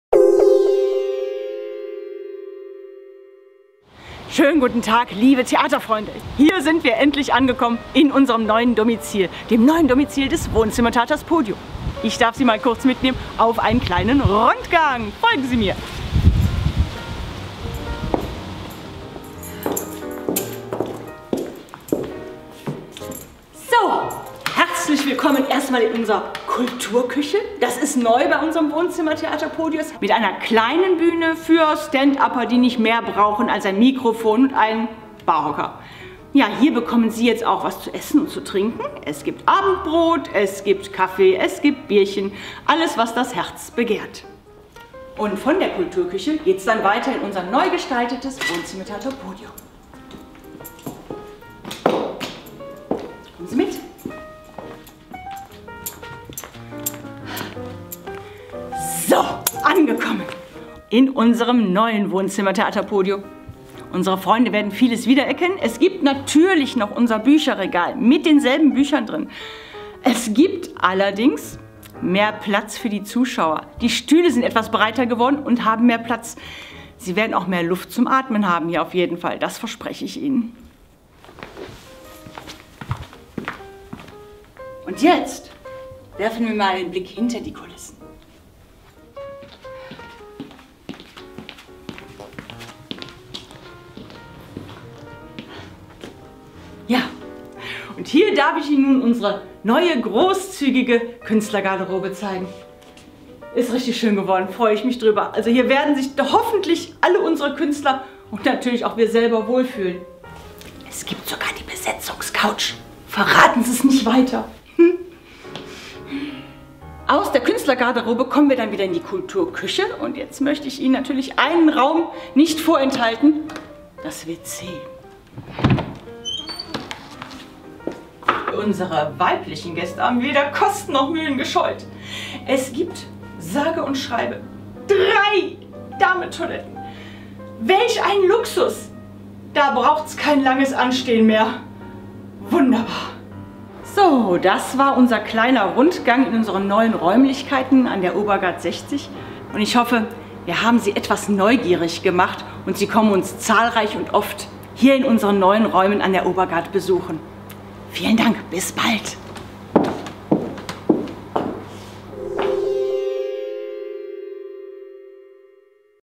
WZ TV zeigt exklusiv das neue Podio-Theater
Ganz große Kleinkunst gibt es ab Freitag im neuen Podio-Theater zu erleben.
Wer schon einen exklusiven Blick in das neu aufgelegte Wohnzimmertheater von
Rüdiger Höfken und Betti Ixkes werfen möchte, bekommt bei WZ TV die
Gelegenheit dazu. Gastgeberin Betti Ixkes führt durch die neuen Räume und
lüftet auch das Geheimnis um die sagenumwobene "Besetzungscouch".